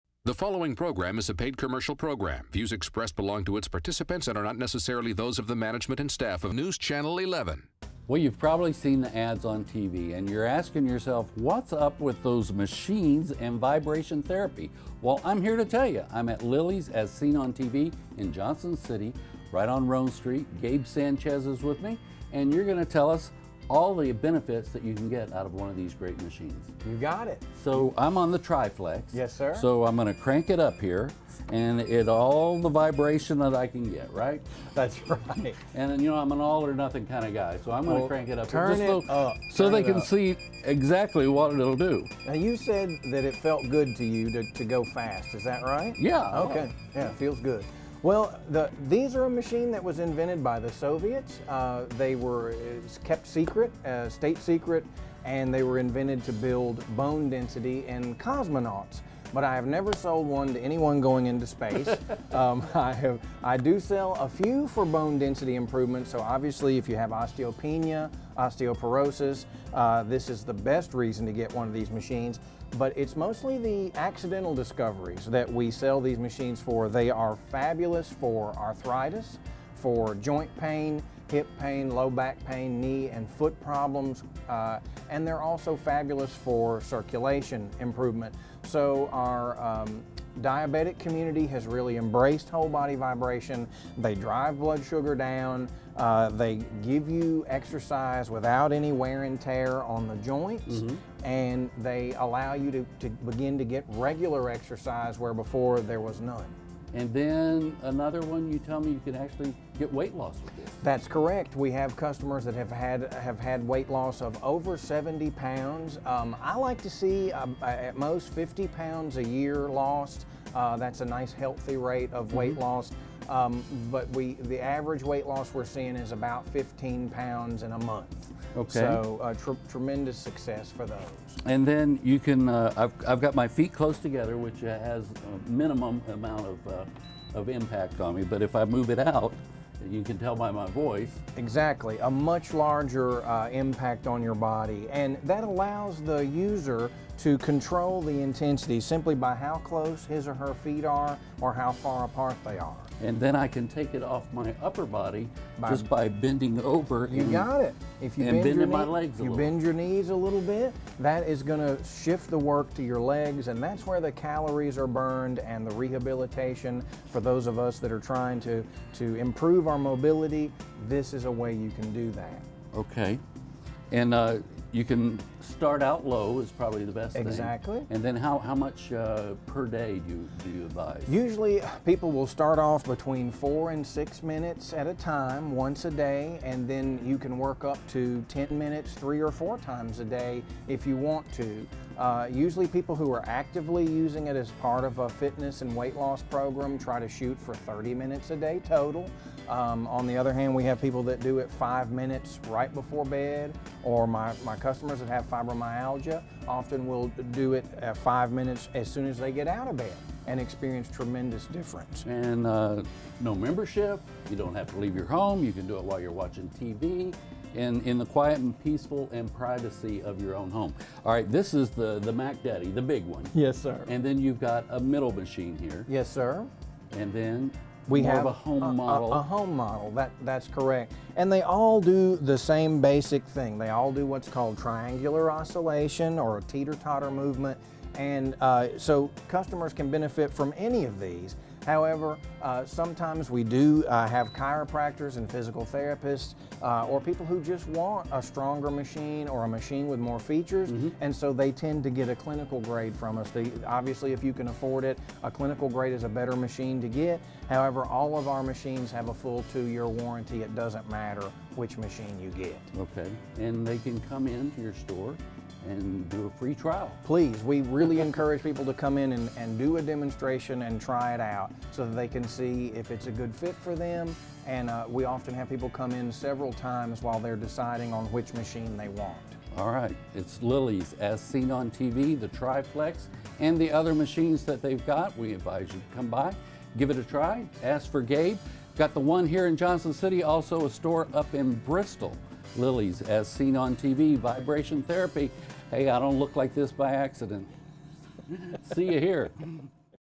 Triflex Vibration Therapy at Lilys As Seen on TV
Morgan King from Daytime Tricities talks with Gabe Sanchez about the benefits of the Tri-Flex.  It is the first two motor, three motion machine of its kind.  Watch as Gabe and Morgan give a demonstration of the Triflex. Go by Lily's As Seen on TV Store in Johnson City, TN and try one for yourself!